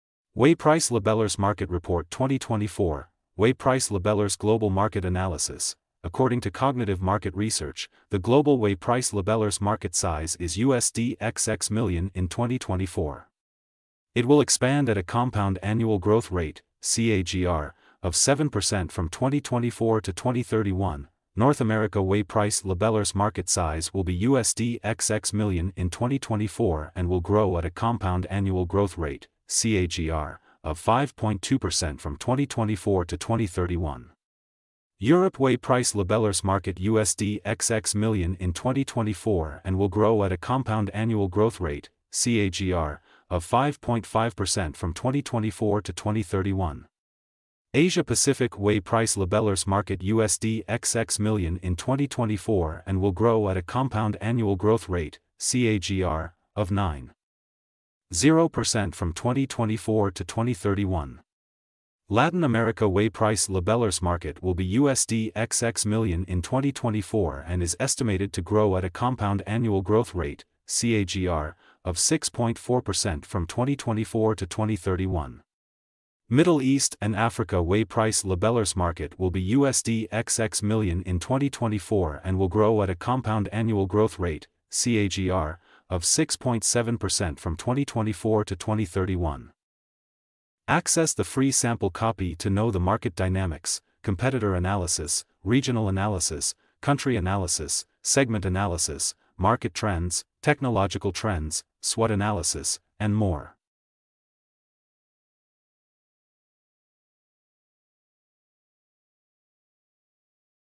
Weigh Price Labellers Market Report 2024
Weigh Price Labellers Market Report 2024 | Forecast, Market Size & Growth

Report Insights: According to Cognitive Market Research, the global Weigh Price Labellers market size is USD XX million in 2024. It will expand at a compound annual growth rate (CAGR) of 7.00% from 2024 to 2031.

North America held the major market share for more than 40% of the global revenue with a market size of USD XX million in 2024 and will grow at a compound annual growth rate (CAGR) of 5.2% from 2024 to 2031.
Europe accounted for a market share of over 30% of the global revenue with a market size of USD XX million.
Asia Pacific held a market share of around 23% of the global revenue with a market size of USD XX million in 2024 and will grow at a compound annual growth rate (CAGR) of 9.0% from 2024 to 2031.
Latin America had a market share for more than 5% of the global revenue with a market size of USD XX million in 2024 and will grow at a compound annual growth rate (CAGR) of 6.4% from 2024 to 2031.
Middle East and Africa had a market share of around 2% of the global revenue and was estimated at a market size of USD XX million in 2024 and will grow at a compound annual growth rate (CAGR) of 6.7% from 2024 to 2031.
The Automatic Weigh Price Labellers held the highest Weigh Price Labellers market revenue share in 2024.